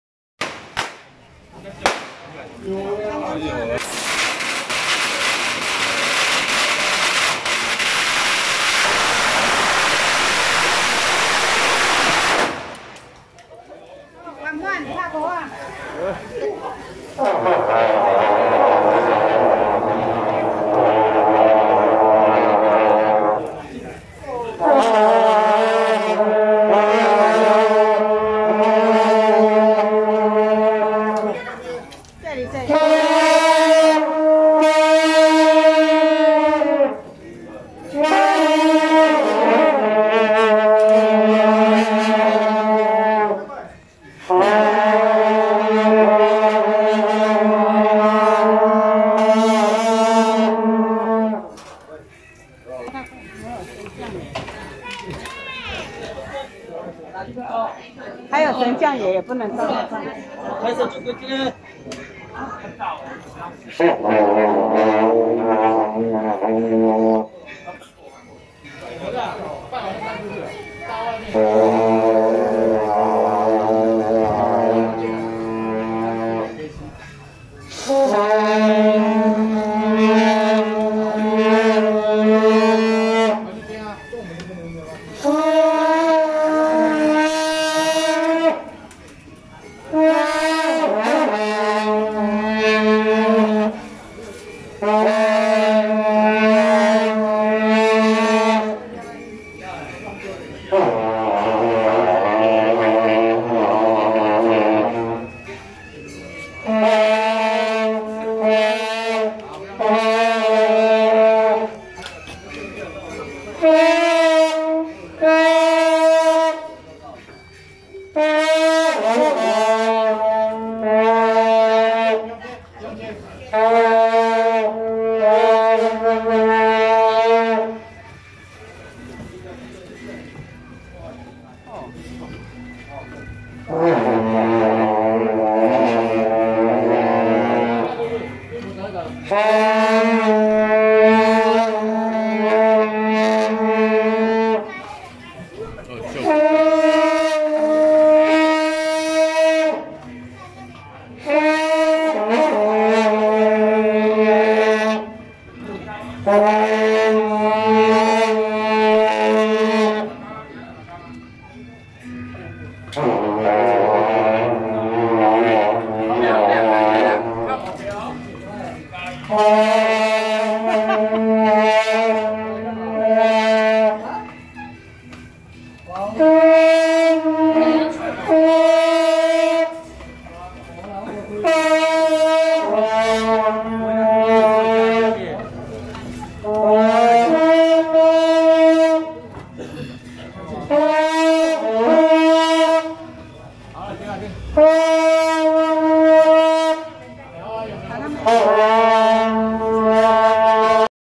【士林湳雅永安社-神將團】~109石牌聖雲宮天上聖母徒步南巡謁祖進香回駕遶境
士林湳雅永安社-神將團